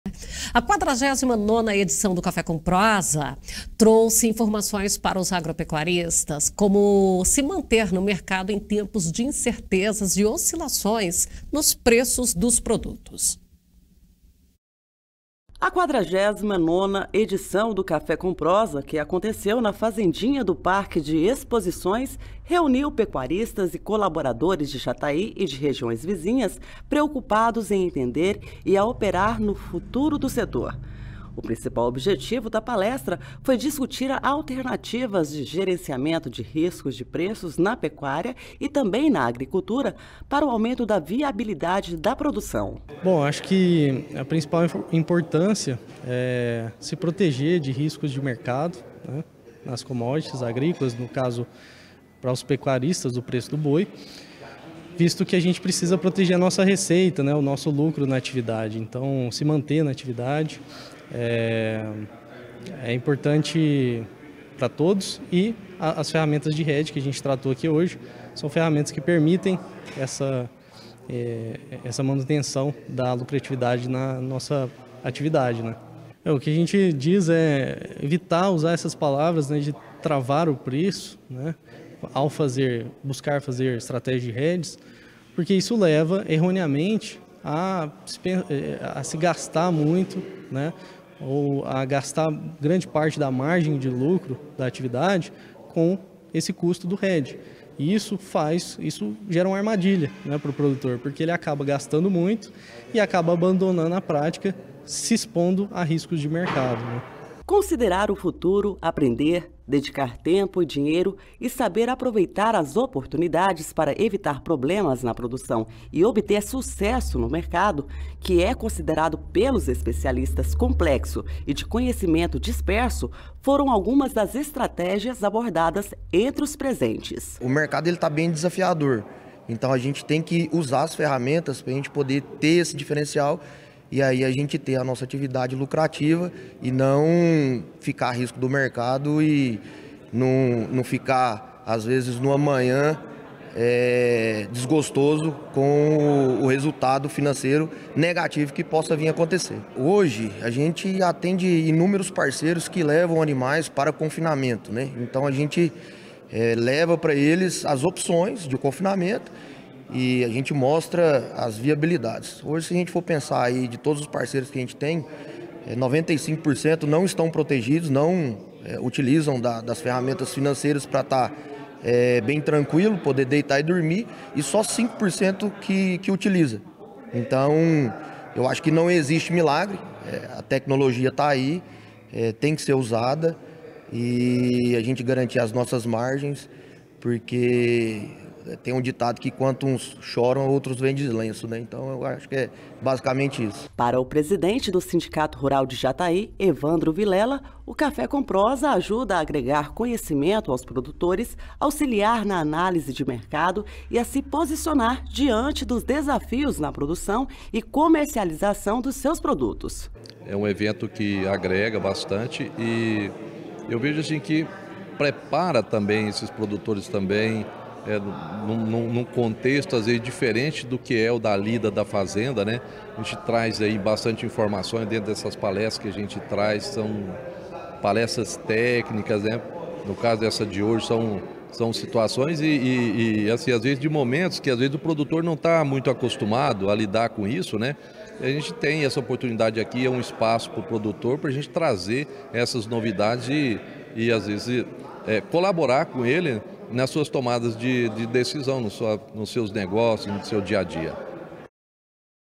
Produtores rurais discutem oscilações nos preços dos produtos
Agropecuaristas recebem orientações de  como se manterem no mercado em tempos de incertezas e oscilações nos preços dos produtos